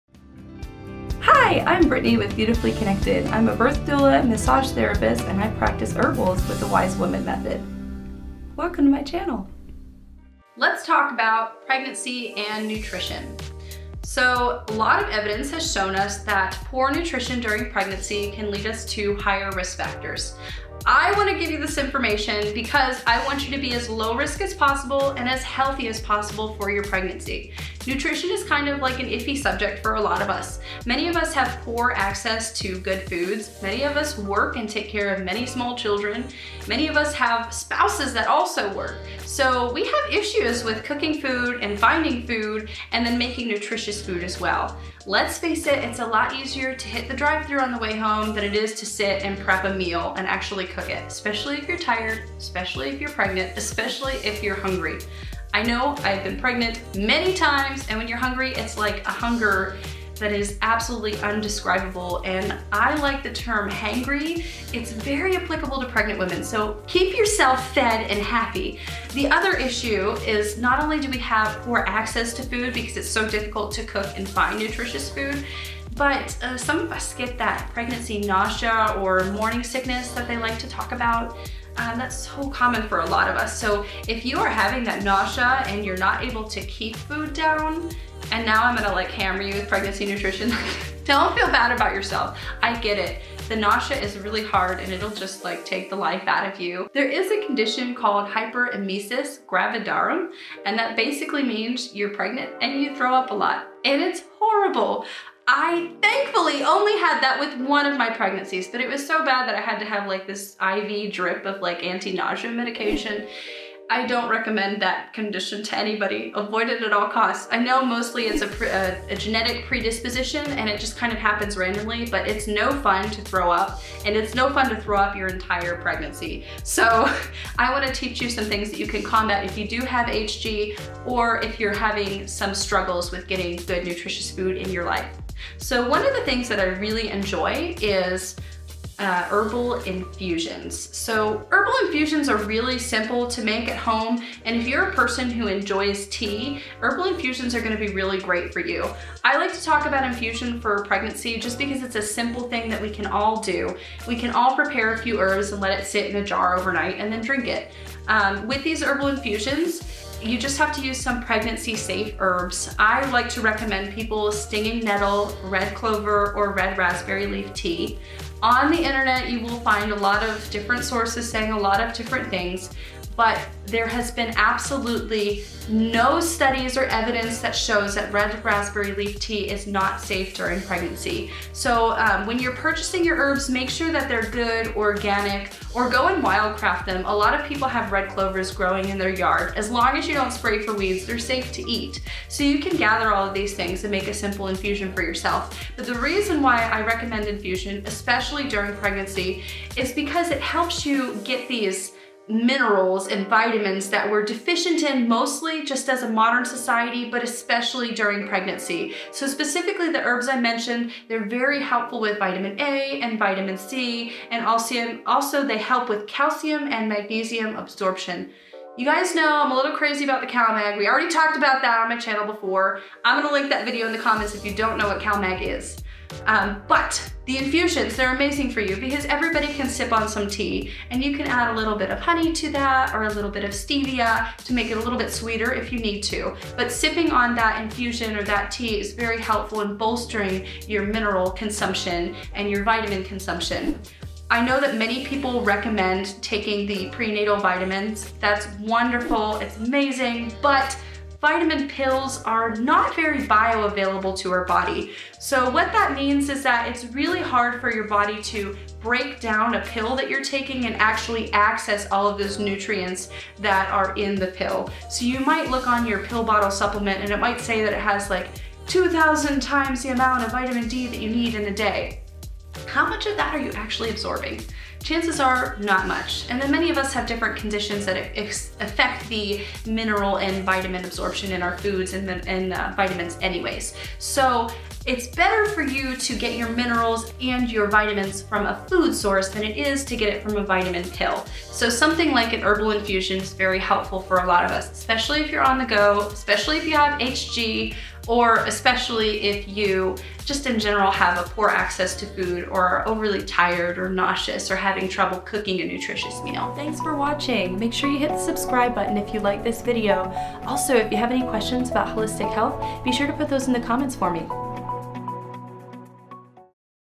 Nutrition with Morning SIckness | Hyperemisis Gravadarium
Hyperemesis Gravadarium or HG is more than just Morning Sickness.

It's horrible. And it's very hard to stay well and nourished while dealing with this condition.

Herbal infusion is one way to stay healthy while pregnant, in general. It is especially helpful while sick!

Use 1 Quart of Hot or Cold Water
2 TBSPs of Herb

I like any of these or a combo of all three!
*Stinging Nettle
*Red Clover
*Red Raspberry Leaf

Why you should take a calcium/magnesium supplement.